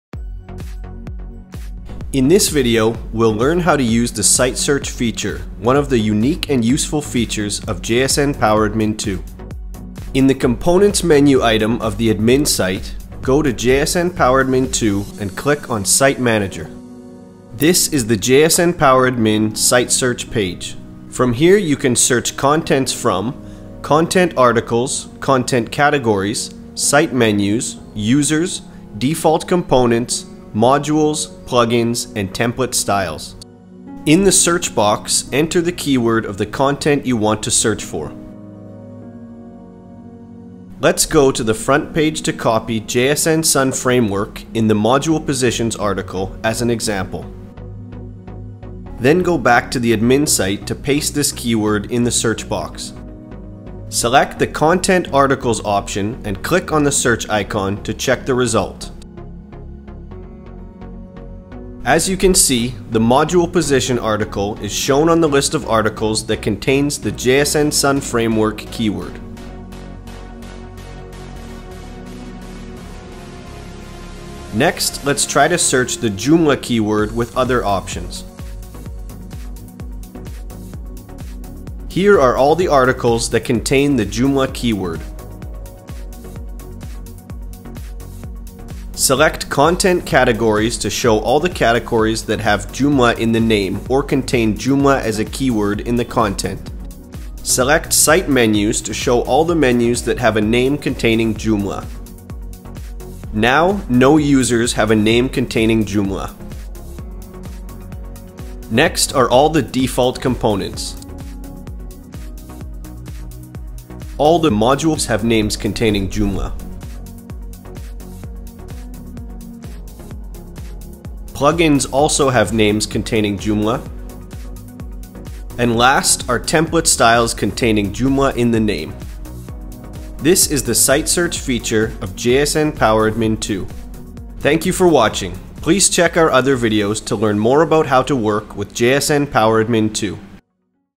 JSN PowerAdmin 2 Tutorials - Video 7: Site Search
In this video, we'll learn how to install JSN PowerAdmin 2 and conduct a quick overview of this extension.